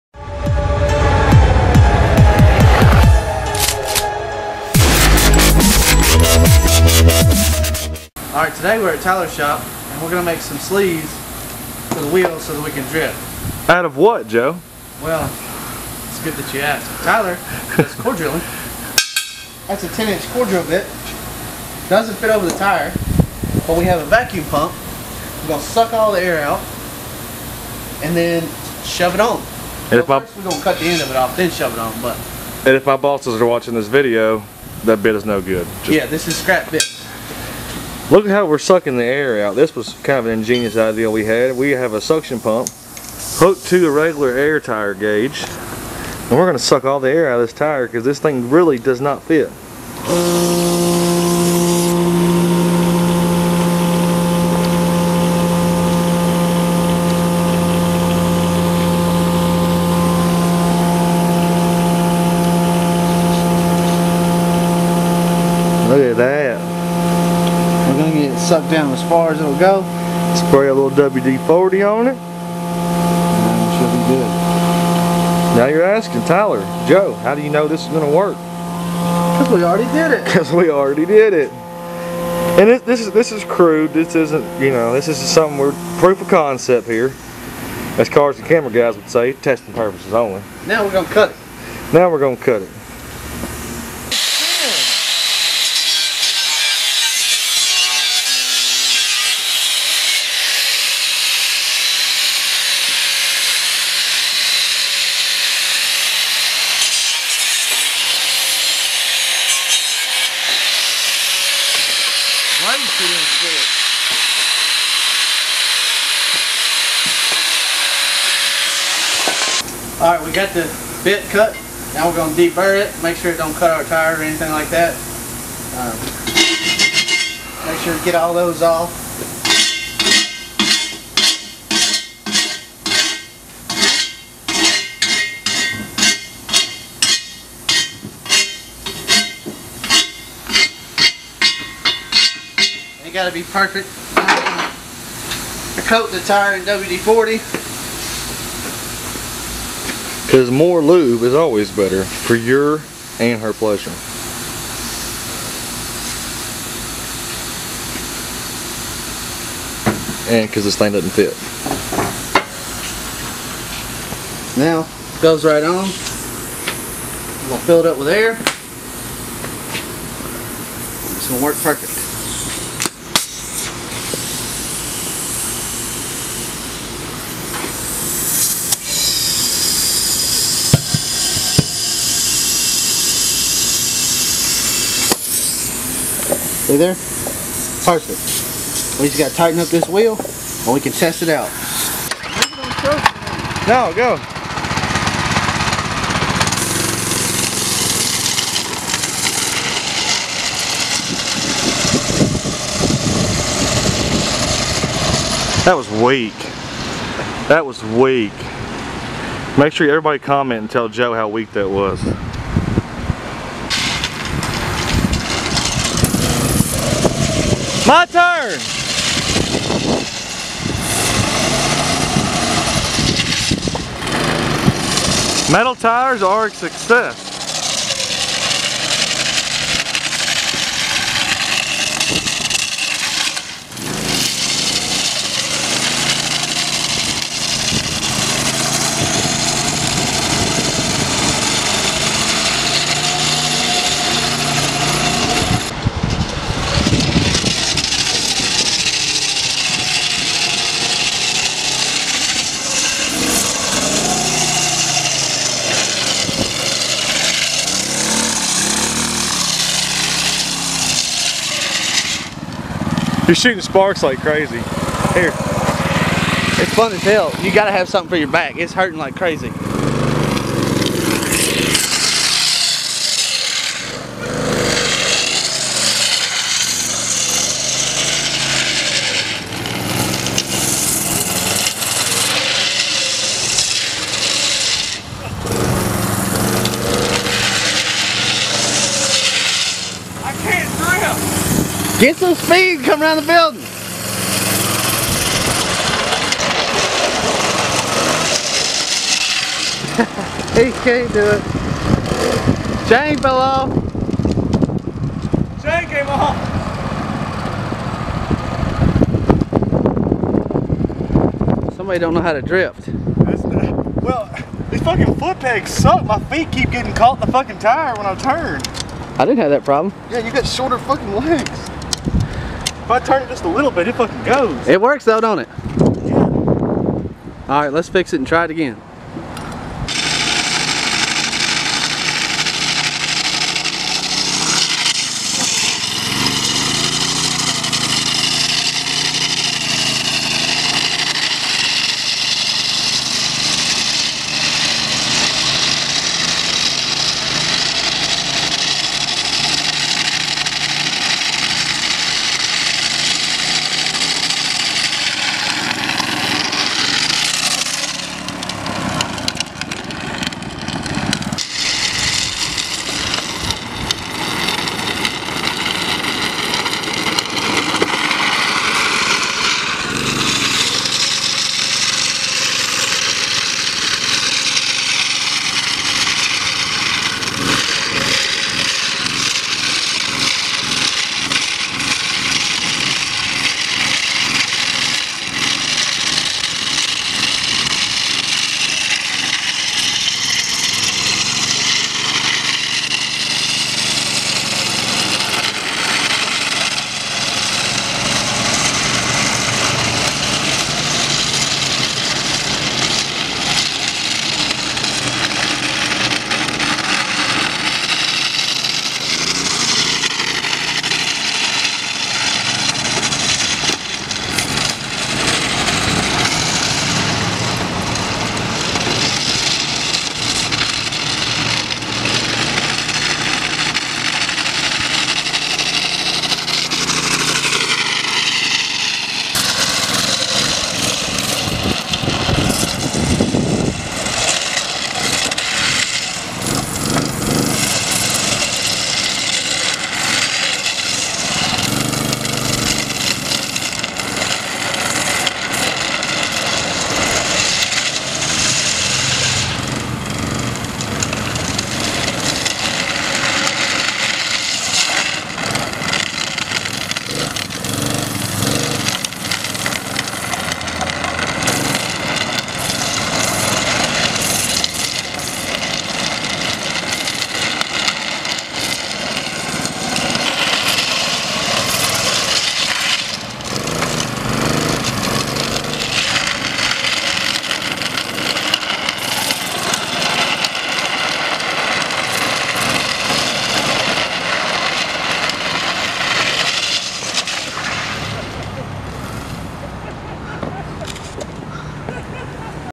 STEEL SLEEVED DRIFT TRIKE!!!! THROWING SPARKS!!!
Testing steel sleeved wheels on our drift trike works good so far.
Still no seat but we have good gas in it this time

trike motor